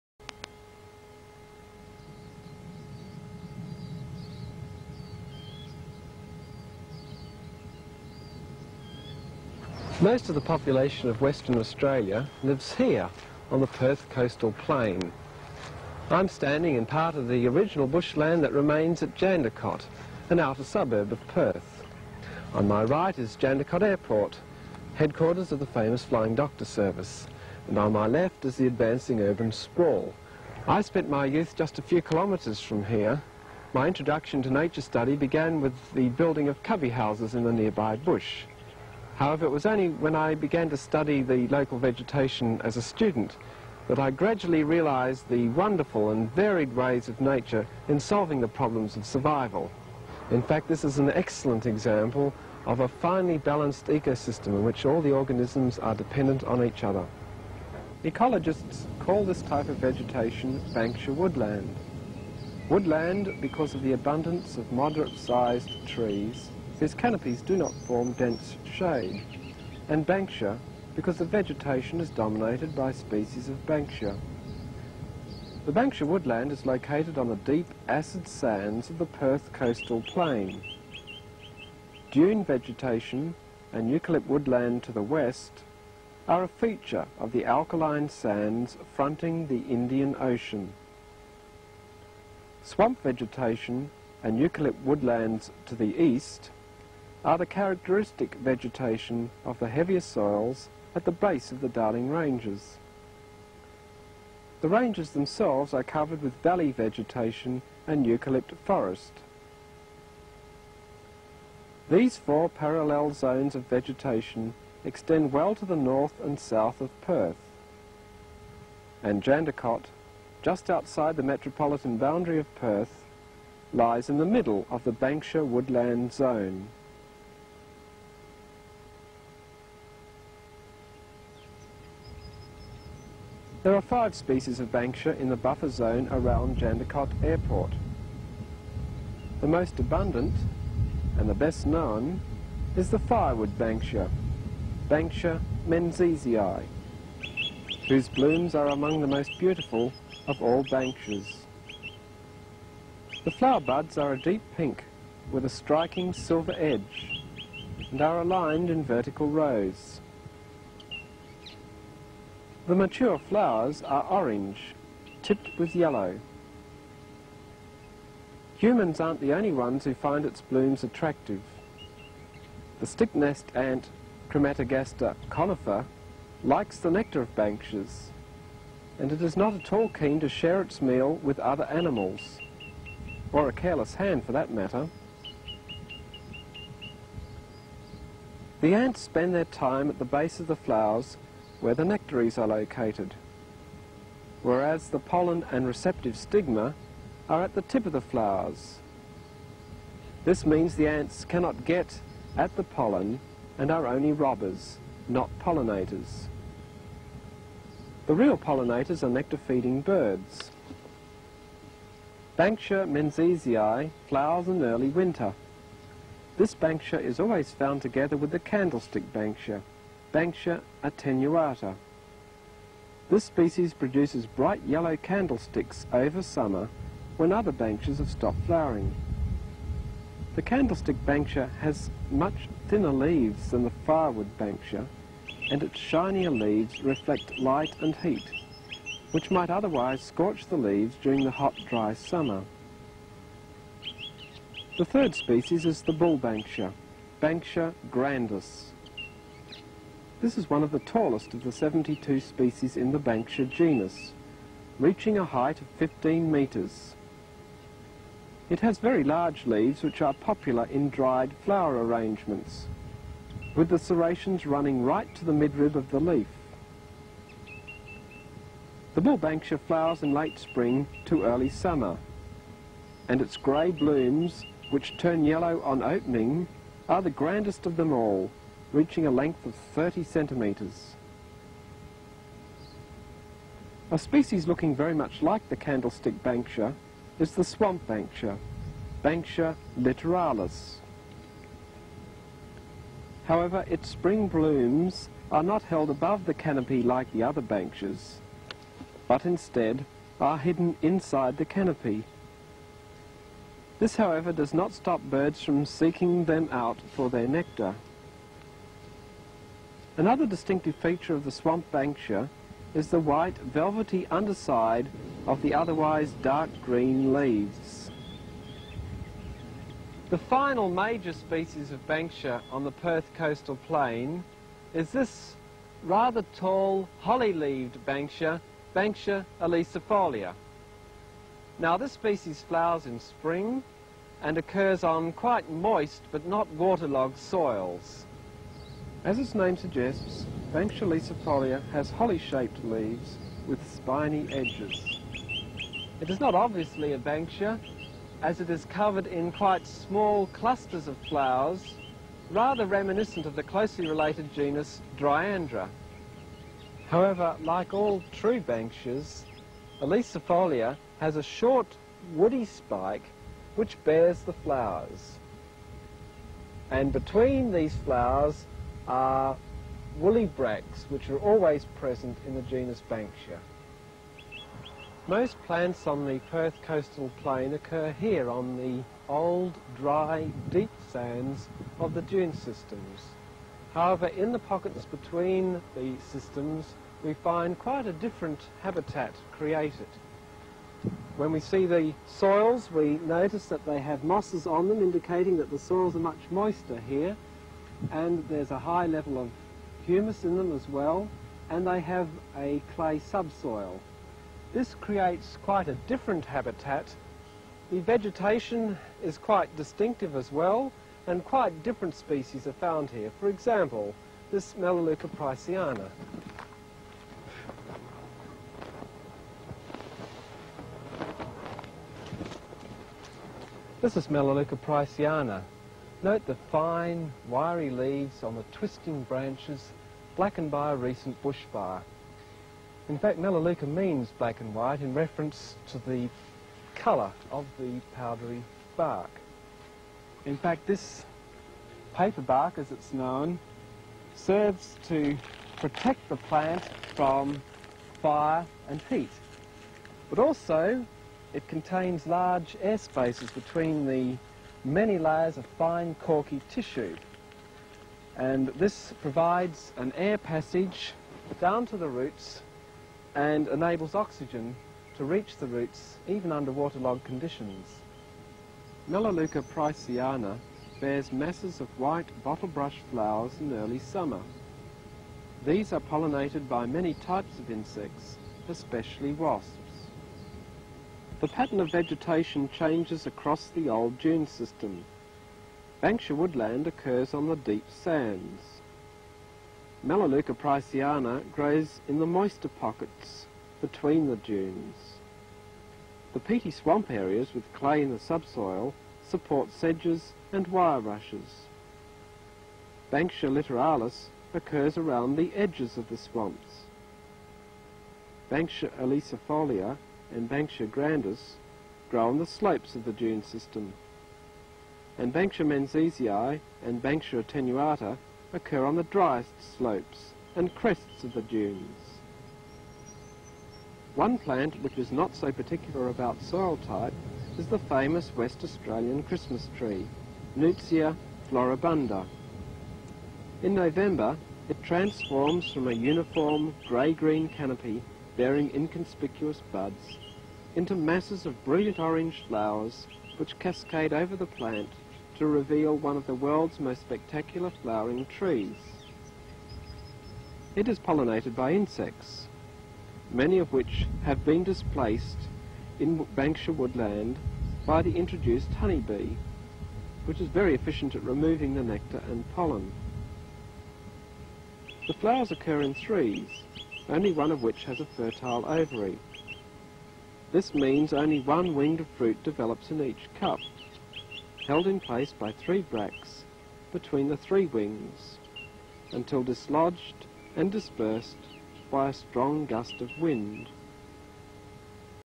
Our Natural Heritage: The Banksia Woodlands part 1
Go back in time (1983) when Jandakot was 'an outer suburb' of Perth.  This video, narrated by Prof Byron Lamont, details the ecology and functional biodiversity of the Banksia woodland sandplain vegeation.  Concepts such as fire adaption, pollination, root morphology are covered.  Dedicated to the late Peter Mioduszewski, amateur botanist, Curtin technician, and good friend.